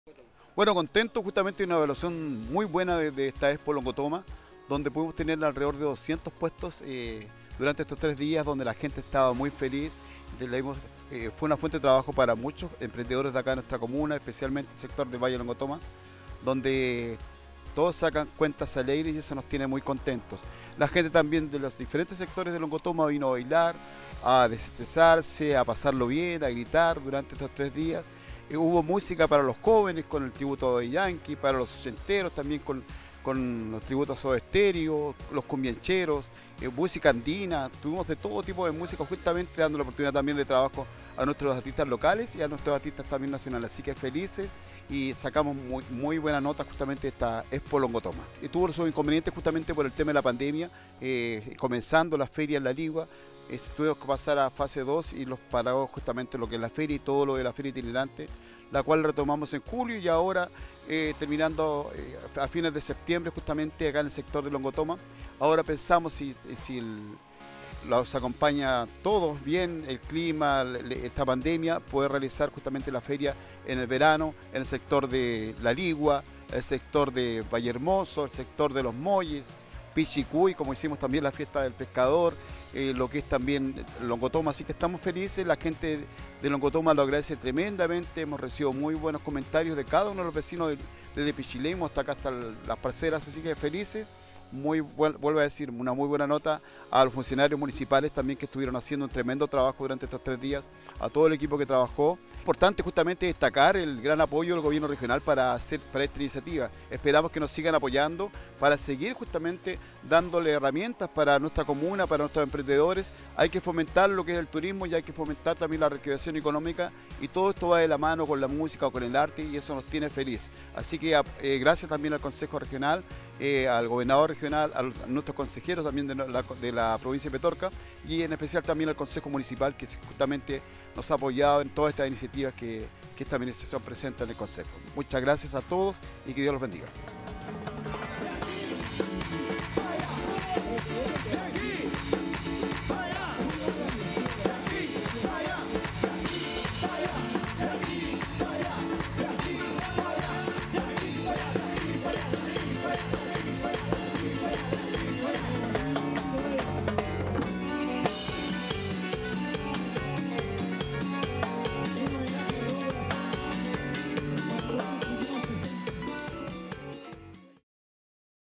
🌺🌸🌼 𝗘𝘅𝗽𝗼 𝗟𝗮 𝗟𝗶𝗴𝘂𝗮 𝟮𝟬𝟮𝟮 𝗜𝘁𝗶𝗻𝗲𝗿𝗮𝗻𝘁𝗲 𝗲𝗻 𝗟𝗼𝗻𝗴𝗼𝘁𝗼𝗺𝗮🌺🌸🌼
🌺🌸🌼 𝗖𝗶𝗲𝗻𝘁𝗼𝘀 𝗱𝗲 𝘃𝗶𝘀𝗶𝘁𝗮𝗻𝘁𝗲𝘀 𝗱𝗶𝘀𝗳𝗿𝘂𝘁𝗮𝗿𝗼𝗻 𝗱𝗲 𝗘𝘅𝗽𝗼 𝗟𝗮 𝗟𝗶𝗴𝘂𝗮 𝟮𝟬𝟮𝟮 𝗲𝗻 𝗟𝗼𝗻𝗴𝗼𝘁𝗼𝗺𝗮🌺🌸🌼
Gran cantidad de personas, provenientes desde diversos puntos de la Región y el País recorrió los distintos stands que fueron parte de 𝗘𝘅𝗽𝗼 𝗟𝗮 𝗟𝗶𝗴𝘂𝗮 𝟮𝟬𝟮𝟮 𝗜𝘁𝗶𝗻𝗲𝗿𝗮𝗻𝘁𝗲 𝗲𝗻 𝗟𝗼𝗻𝗴𝗼𝘁𝗼𝗺𝗮, 𝗺𝘂𝗲𝘀𝘁𝗿𝗮 𝗼𝗿𝗴𝗮𝗻𝗶𝘇𝗮𝗱𝗮 𝗽𝗼𝗿 𝗹𝗮 𝗠𝘂𝗻𝗶𝗰𝗶𝗽𝗮𝗹𝗶𝗱𝗮𝗱 𝗱𝗲 𝗟𝗮 𝗟𝗶𝗴𝘂𝗮 𝗹𝗼𝘀 𝗱𝗶́𝗮𝘀 𝟯𝟬 𝗱𝗲 𝘀𝗲𝗽𝘁𝗶𝗲𝗺𝗯𝗿𝗲, 𝟬𝟭 𝘆 𝟬𝟮 𝗱𝗲 𝗼𝗰𝘁𝘂𝗯𝗿𝗲,  para promover las actividades productivas y comerciales de la Comuna. El evento concitó la atracción de turistas que llegaron en gran número, provenientes de diferentes puntos del Valle Central, de las regiones de Valparaíso, Coquimbo y Santiago, que aprovecharon el buen fin de semana vivido en la zona, a pesar de algunos chubascos menores y las bajas temperaturas.🤗
El jefe comunal Patricio Pallares Valenzuela agradeció a los funcionarios municipales por el importante despliegue realizado para poner a funcionar este evento, así como al Consejo Regional, al Gobernador y a los concejales de laligua por aportar los recursos necesarios para el éxito de la Feria.🤩🎙🎡
📲 Revisen el video  😉